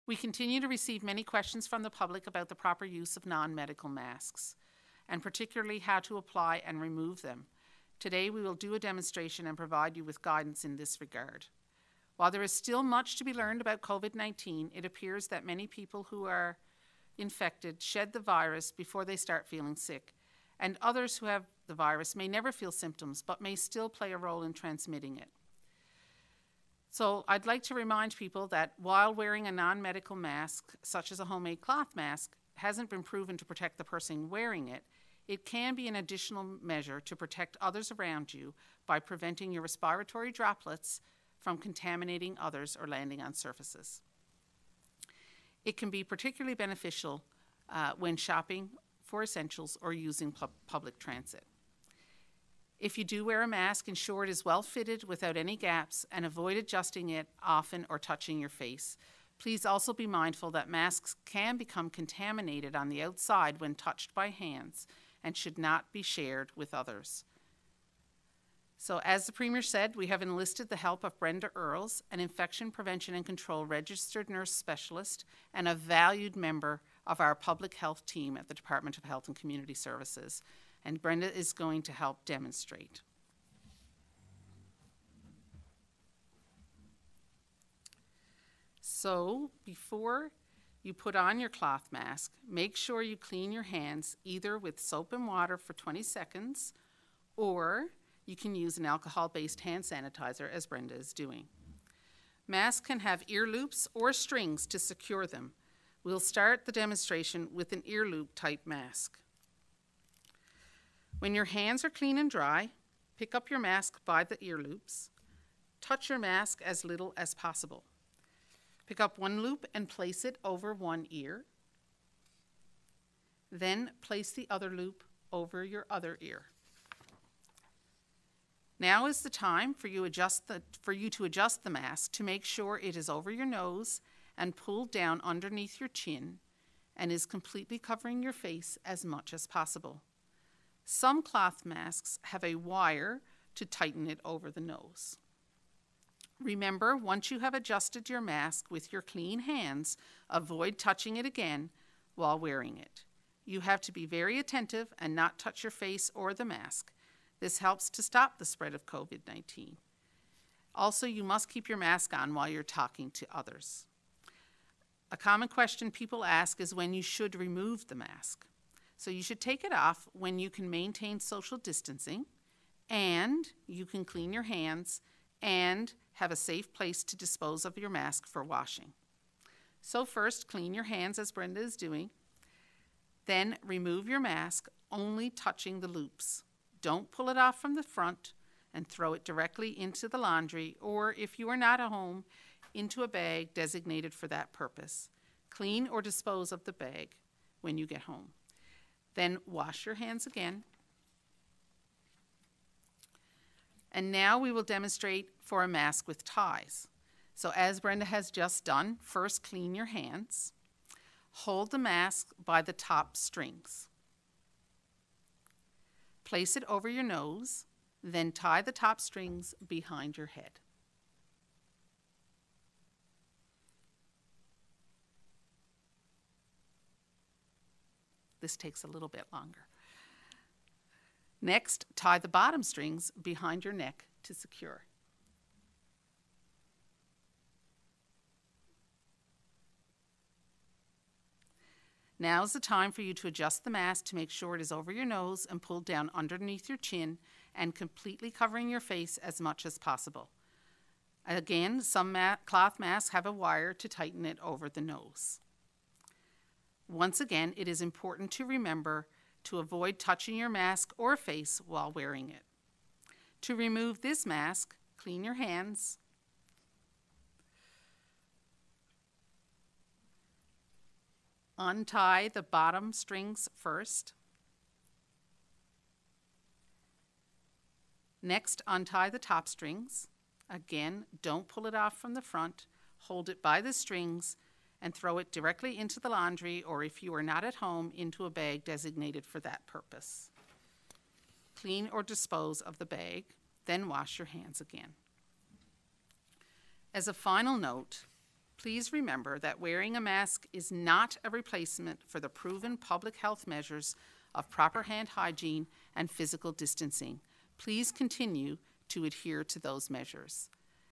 Mask Safety Demo
Brenda Earles (Infection Control Nurse Specialist with Health and Community Services) joined our news conference on Tuesday, April 21, to demonstrate how to safely wear and remove homemade and non-medical face masks. Thank you Brenda! Check it out. 😷